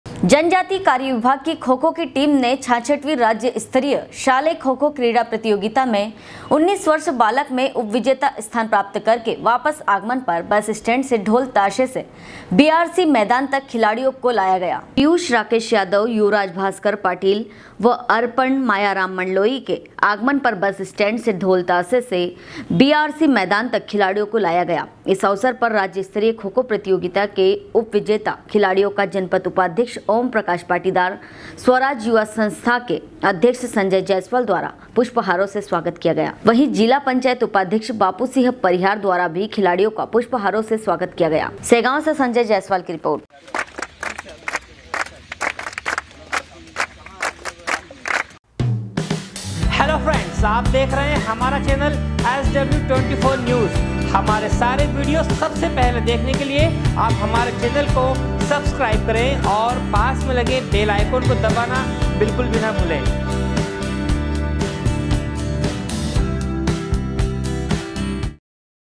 जनजाति कार्य विभाग कि खो खो की टीम ने 66 वी राज्य स्तरीय शालेय खो खो क्रीडा प्रतियोगिता मैं 19 वर्ष
जनजाति कार्य विभाग कि खो खो की टीम ने 66 वी राज्य स्तरीय शालेय खो खो क्रीडा प्रतियोगिता मैं 19 वर्ष बालक मे उपविजेता स्थान प्राप्त करके वापस आगमन पर बस स्टैंड से ढोल ताशे से बीआरसी मैदान तक खिलाड़ियों को लाया गया।
सेगांव से संजय जायसवाल की रिपोर्ट।